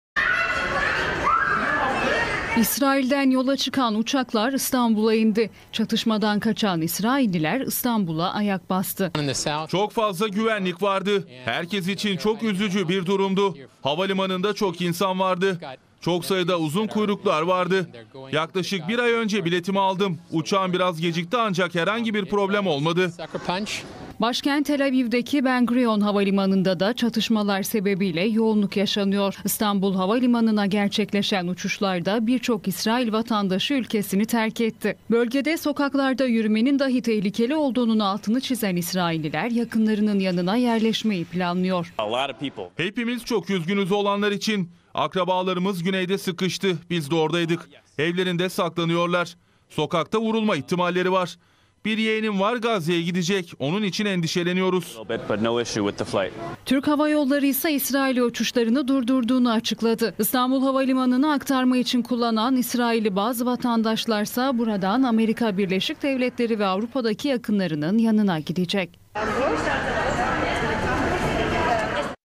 Hamas-İsrail Arasındaki Çatışmadan Kaçan İsrailliler Türkiye'ye Geldi!
Hamas-İsrail arasındaki çatışma devam ederken, İsrail vatandaşlarını taşıyan uçaklar Tel Aviv'den İstanbul Havalimanı'na iniş yaptı. Oradaki durumu anlatan İsrail vatandaşları buradan Amerika ve Avrupa'daki yakınlarının yanına gidecek.